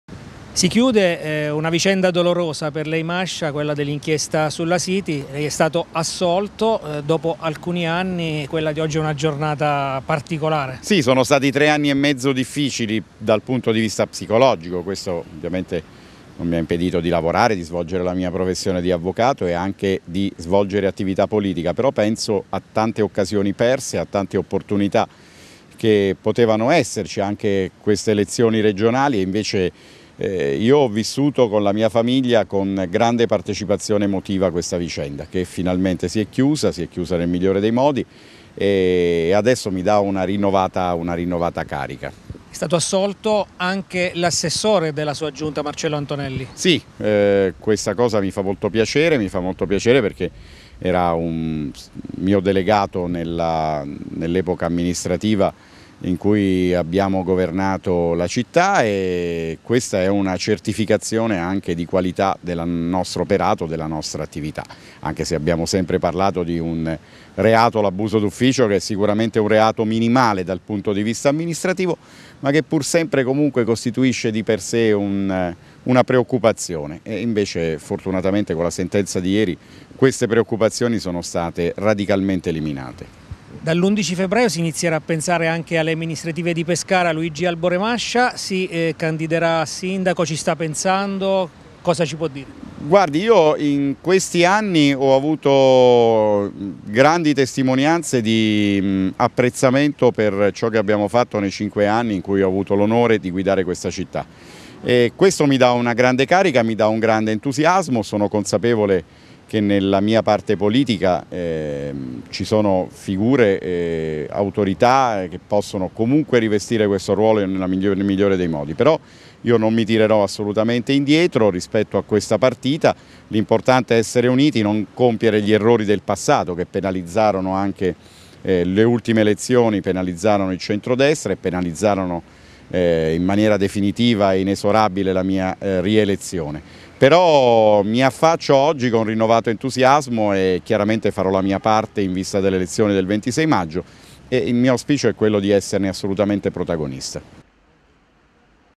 Inchiesta La City, assolti Mascia e Antonelli
Nel processo sulla costruzione della City sono stati assolti l'ex sindaco di Pescara Luigi Albore Mascia e Marcello Antonelli, a quell'epoca assessore comunale. Sono invece stati rinviati a giudizio 12 indagati che compariranno per la prima udienza l’8 maggio. Mascia ora pensa ad una ricandidatura a sindaco